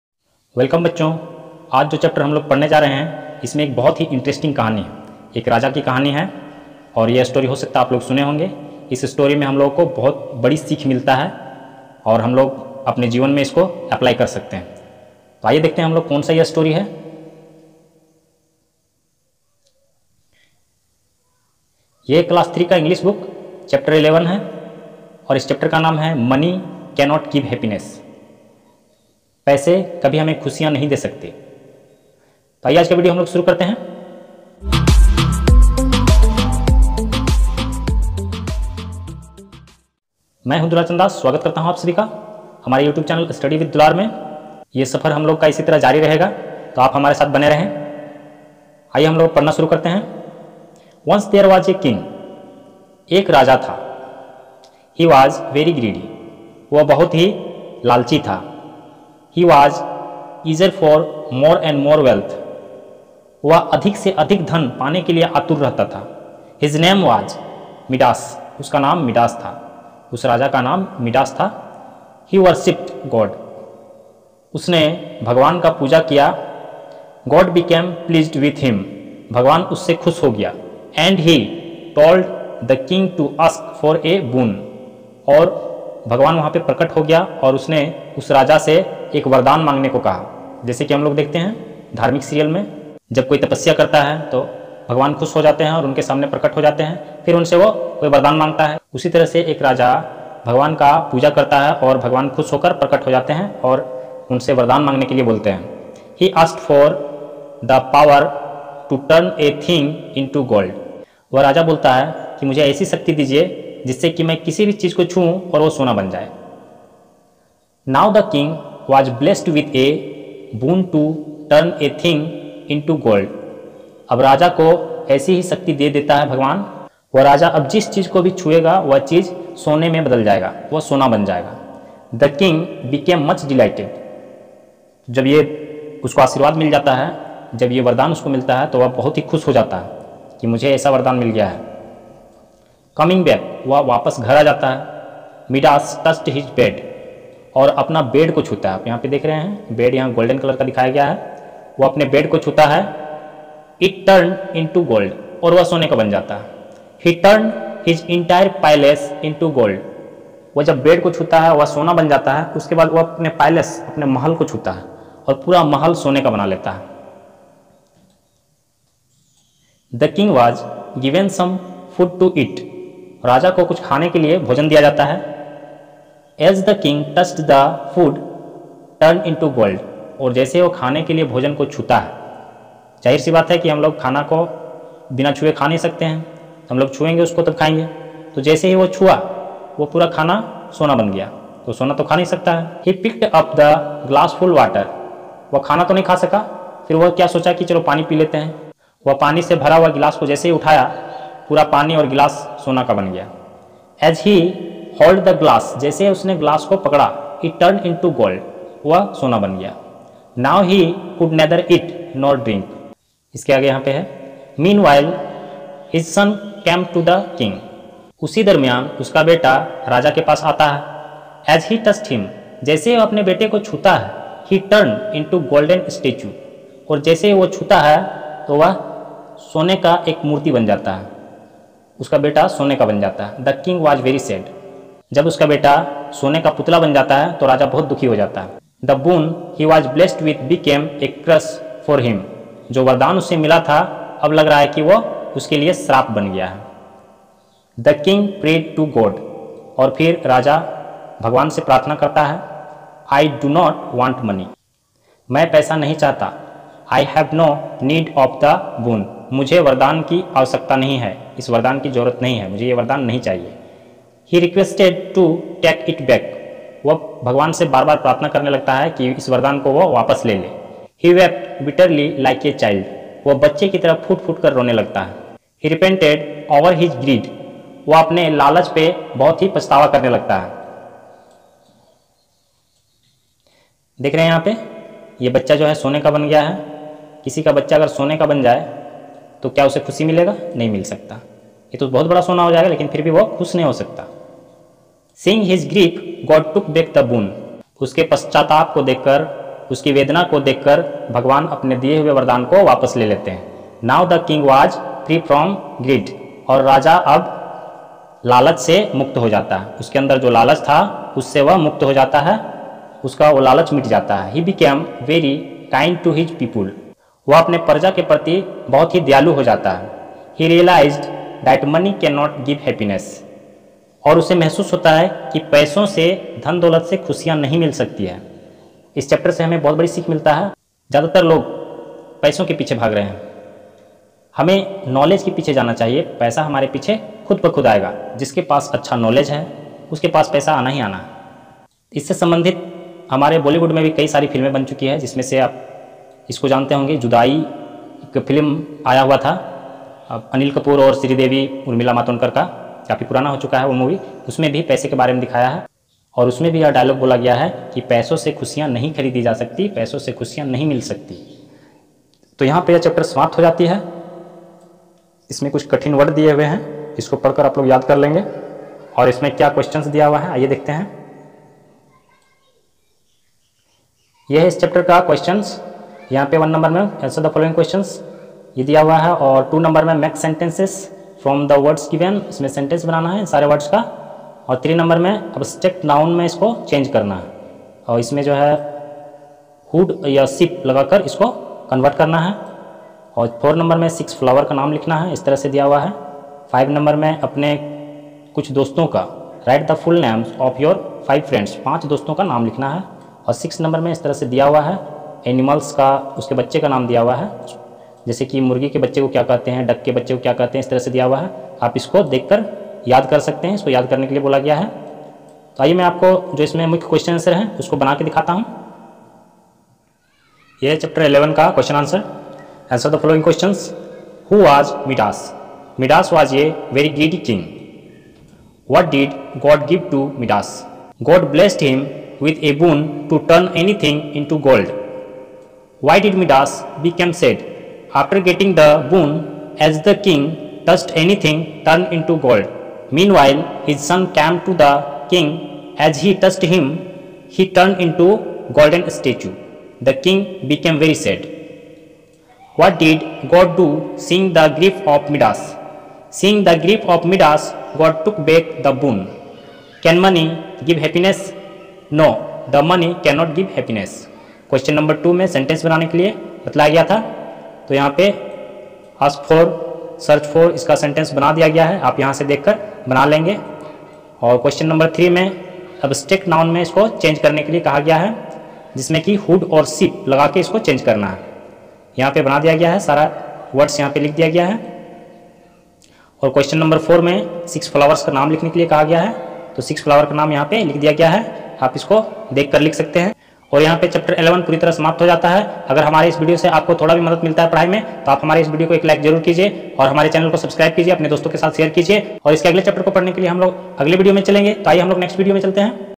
Money cannot give happiness Ch 11 Eng Class 3 Sarswati Shishu Vidya Mandir सरस्वती शिशु विद्या मंदिर
नमस्कार
बच्चों इस विडियो में क्लास 3 इंग्लिश चेप्टर 11 की स्टडी करवाया गया है और उसके प्रश्न उत्तर भी करवाया गया है। आप सभी इसे ध्यान पूर्वक देखें और इसके प्रश्न उत्तर भी याद करें।